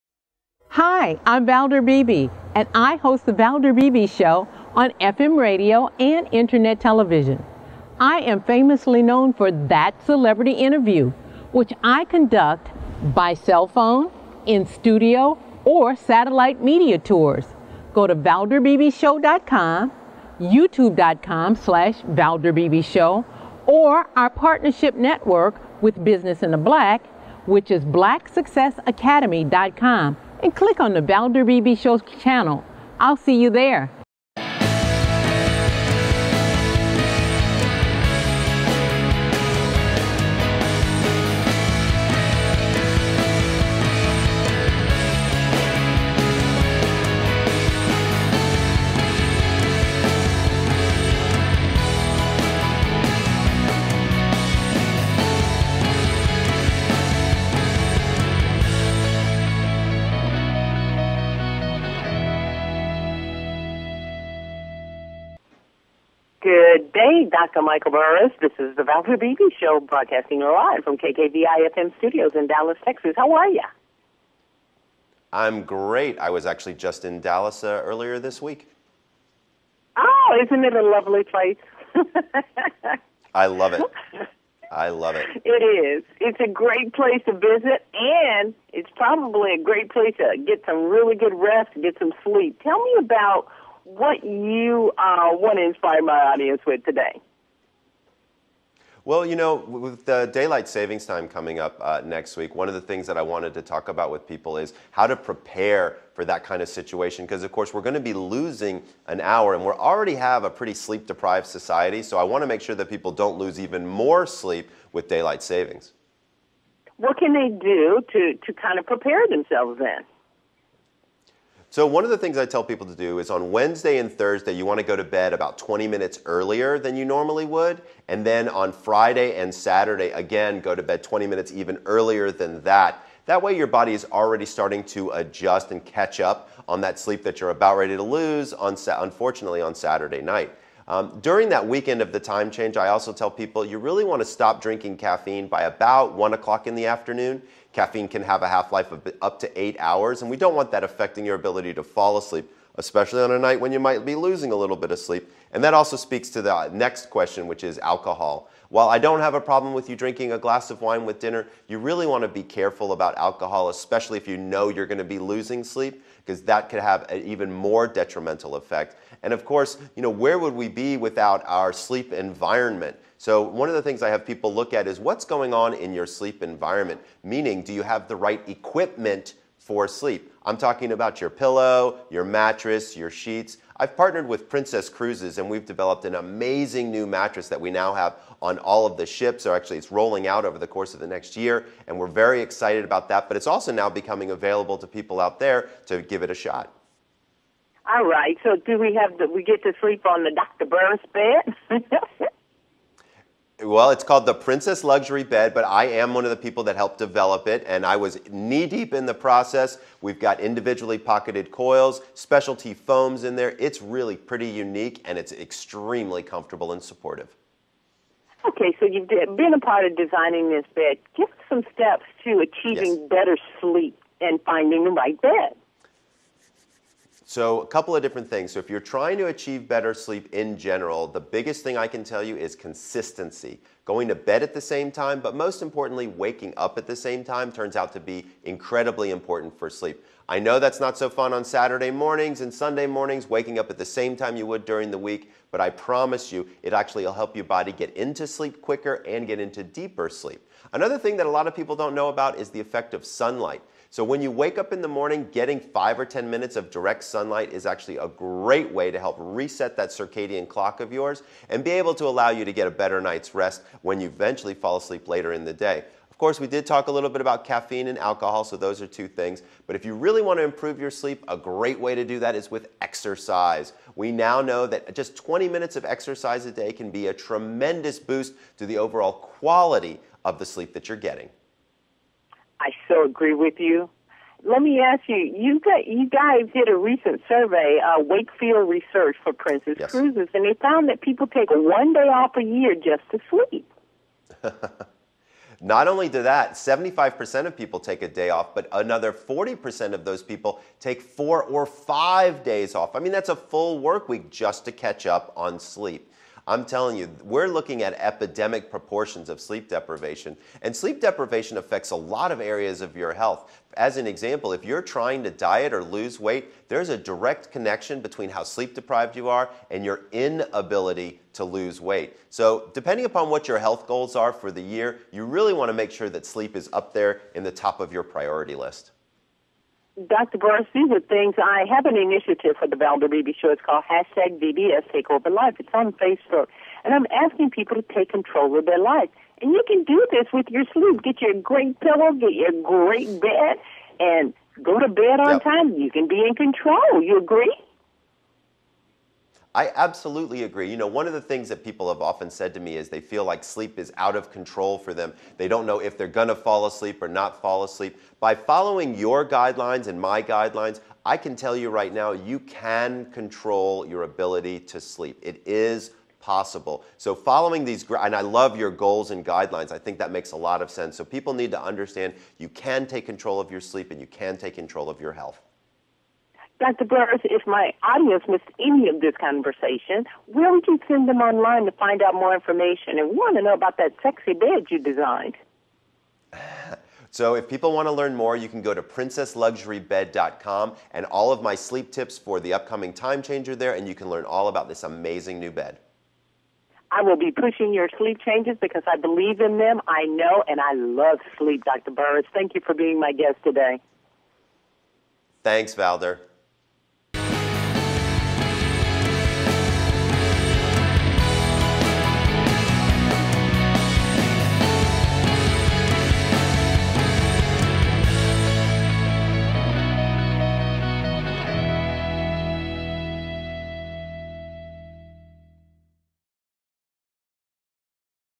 Sleep Doctor Dr Michael Breus
Daylight saving time sleep tips with Dr. Michael Breus "The Sleep Doctor"